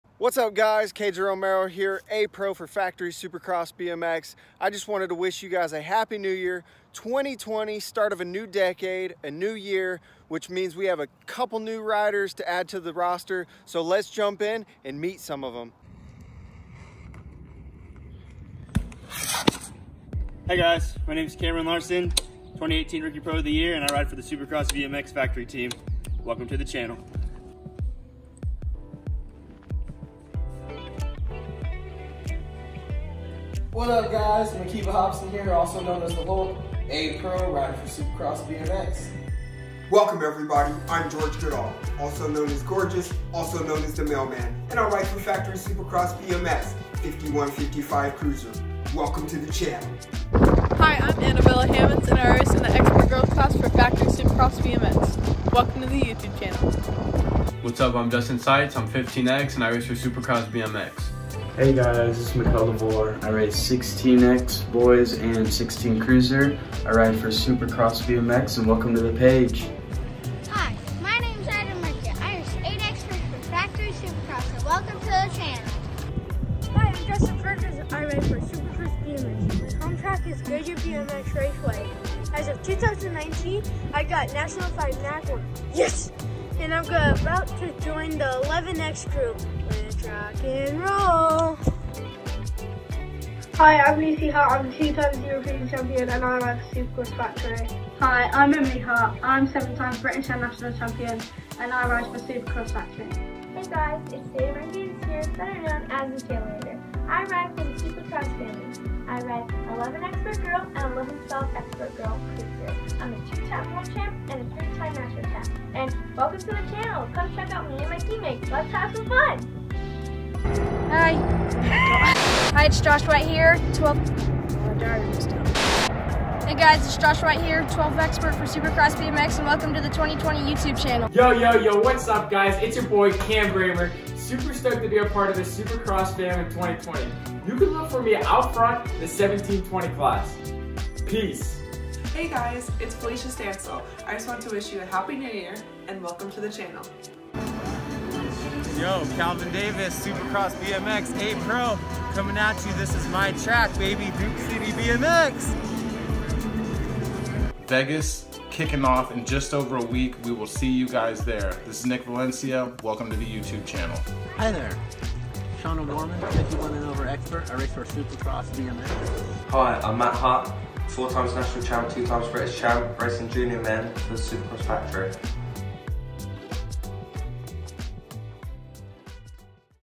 Meet the 2020 Factory Supercross BMX Riders
Happy New Years everyone! To bring in a new season, we wanted to introduce you guys to some new faces on the Factory Supercross BMX team. So we put together a video of our 2020 team roaster. You'll see a lot familiar faces and some new faces.

We are stoked for the season to kick off next weekend in Las Vegas! If you're going to Vegas, makes sure to stop by the Supercross pit to grab a few stickers and meet the riders! See you there!

Not every rider was able to submit a video. So for a full team listing. Please visit our team page at :